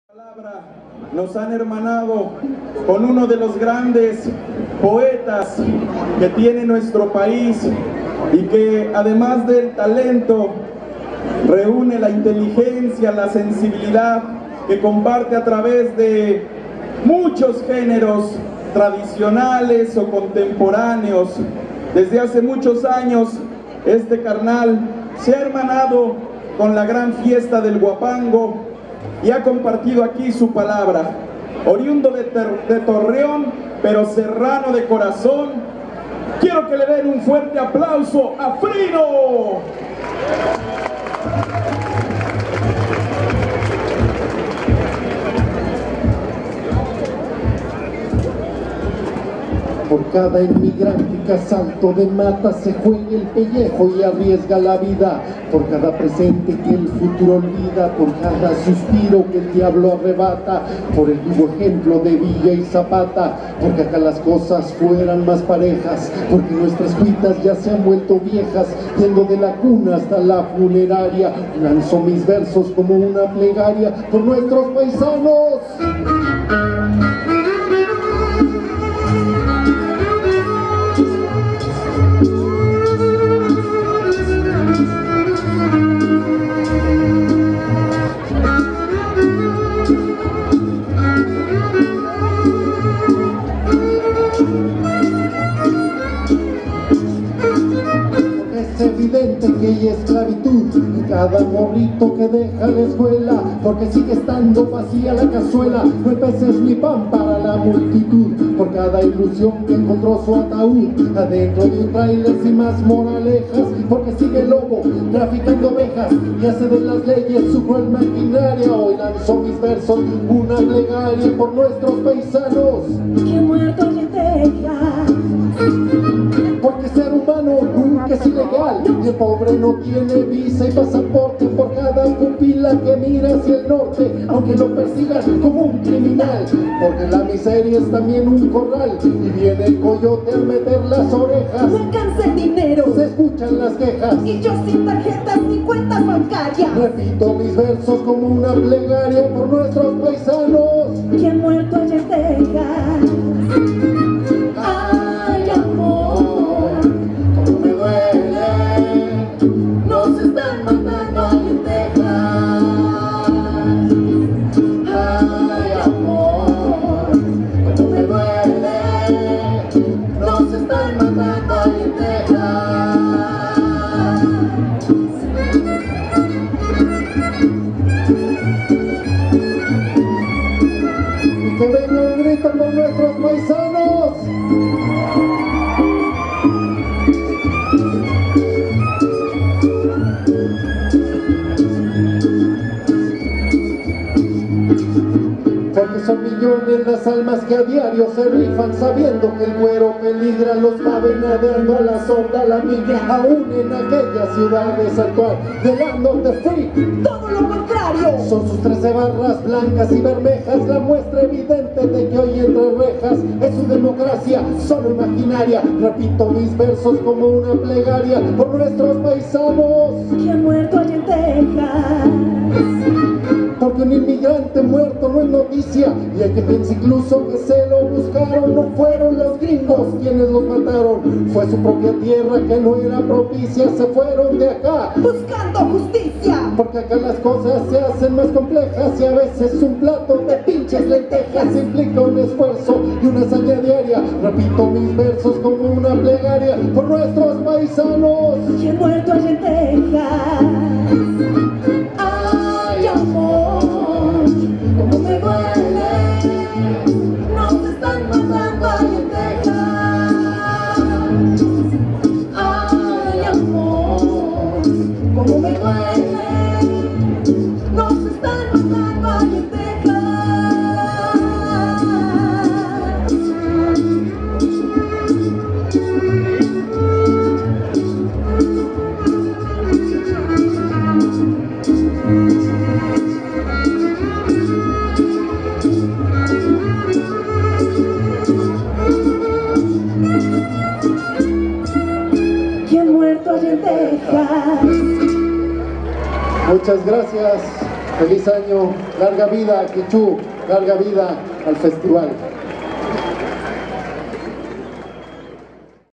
FRINO.XICHÚ,GUANAJUATO.DICIEMBRE 30 DE 2019
Participación de Frino en el 37 Festival de Huapango Arribeño y la Cultura de la Sierra Gorda, en Xichú Estado de Guanajuato, el 30 de diciembre de 2019